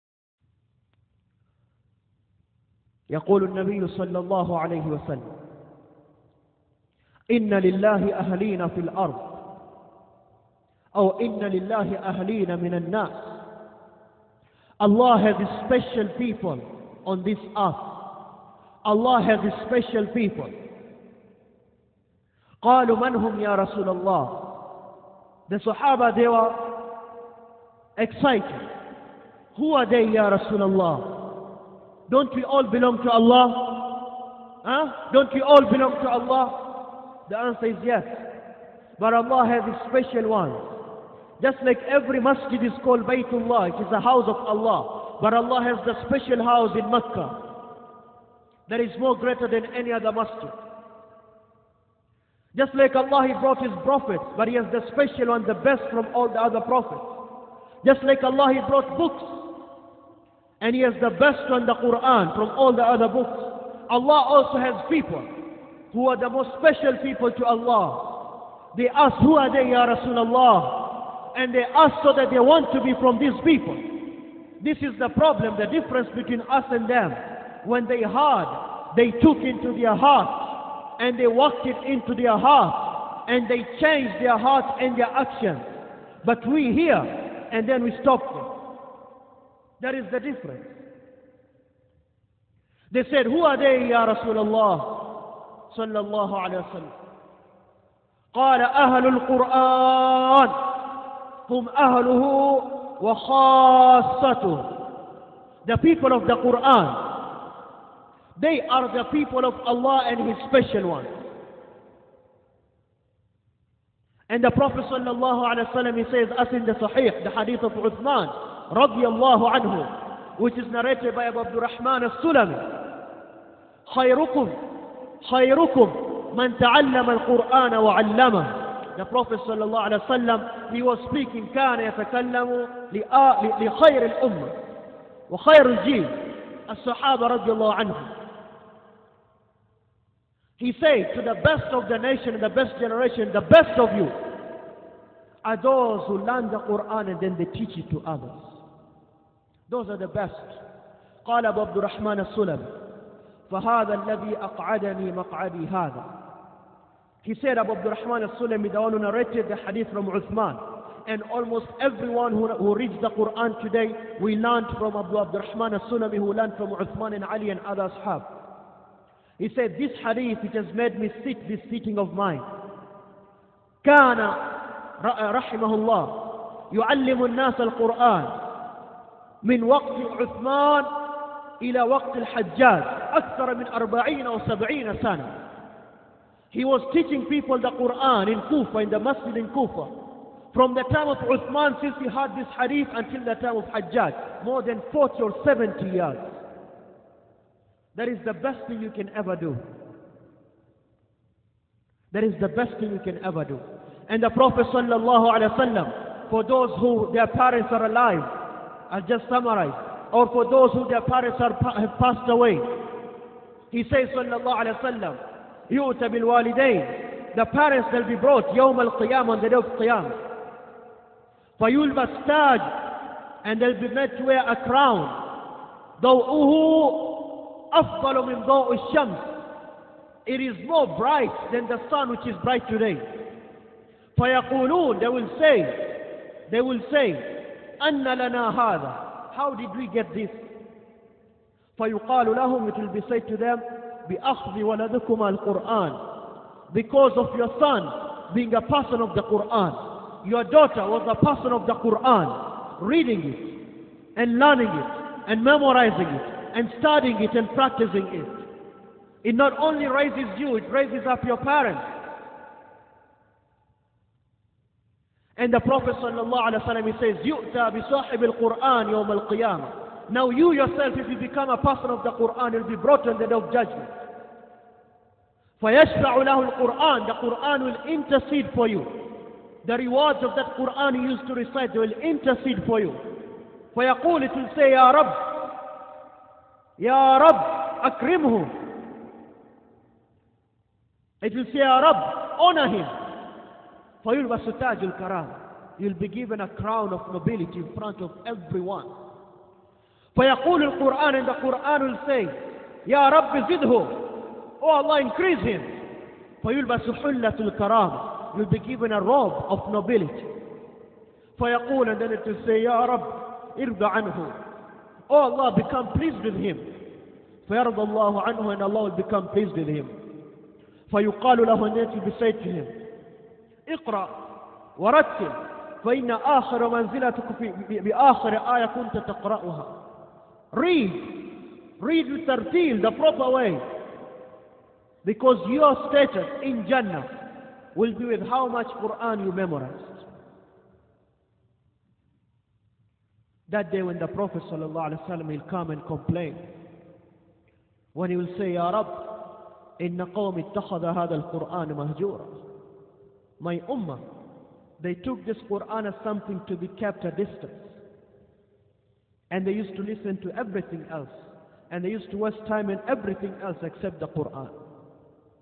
Some of the Excellences of learning Qur'an: By Sheikh Abu Umar AbdulAziz
Special people in front of Allah. Some of the Excellences of learning Qur'an: By Sheikh Abu Umar AbdulAziz